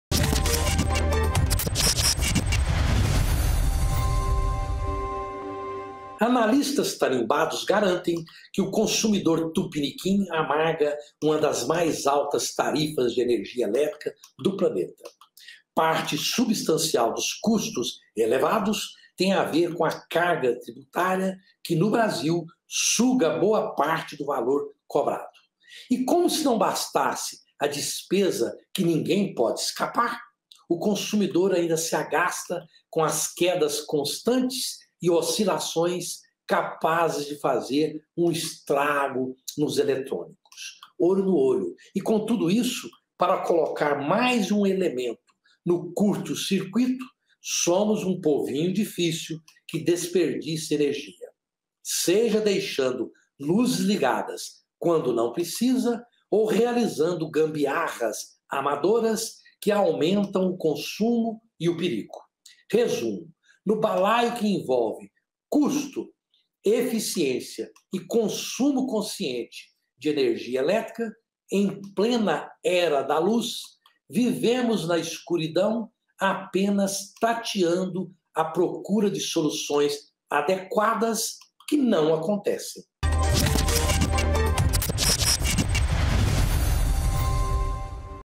OLHO NO OLHO: O CURTO CIRCUITO
HOJE NO QUADRO OLHO NO OLHO O JORNALISTA FALA SOBRE AS PESSOAS QUE INSISTEM EM DESPERDIÇAR ENERGIA E TAMBÉM COMENTA SOBRE A CARGA TRIBUTÁRIA QUE ELEVA O PREÇO DA TARIFA DE ENERGIA NO PAÍS.